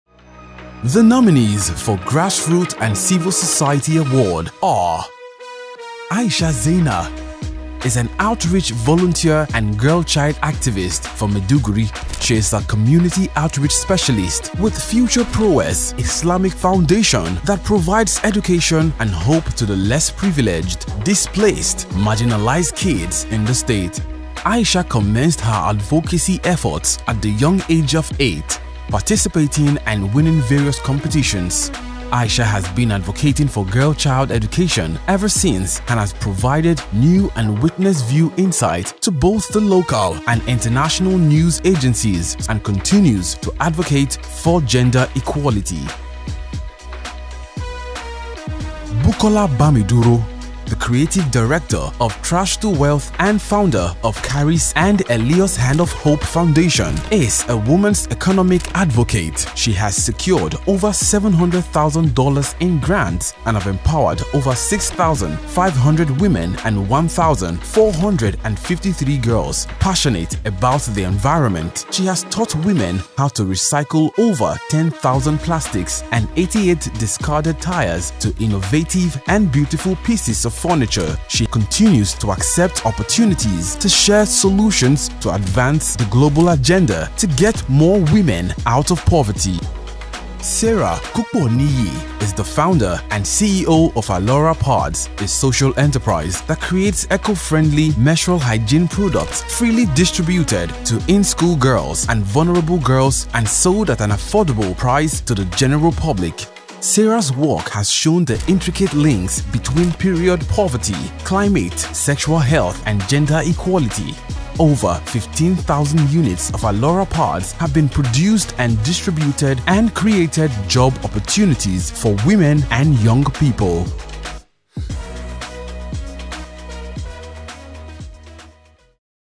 Nominees for Grassroots and Civil Society
The world is a better place because of those who push boundaries and make change. The International Women's Day Awards Gala 2023 celebrates these leaders and their efforts to promote gender equality.

The Nominees for Grassroots and Civil Society are Aisha Zannah, Sarah Kuponiyi, and Bukola Bamiduro.